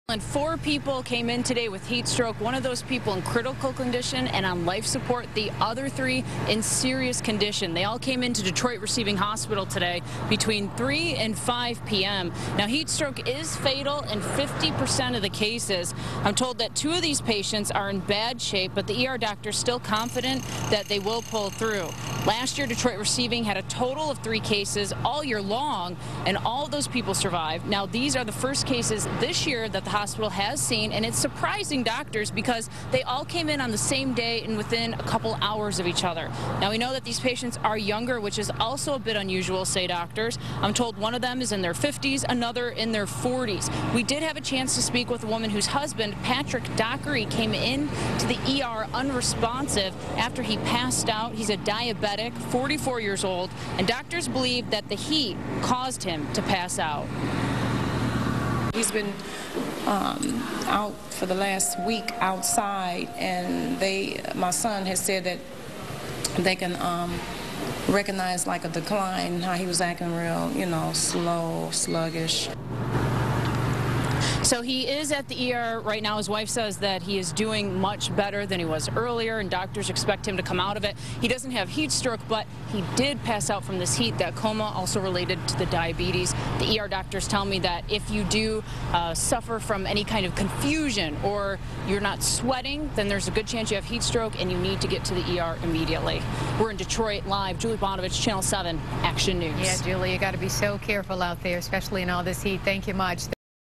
Four people being treated
Four people are being treated for heat stroke at the DMC.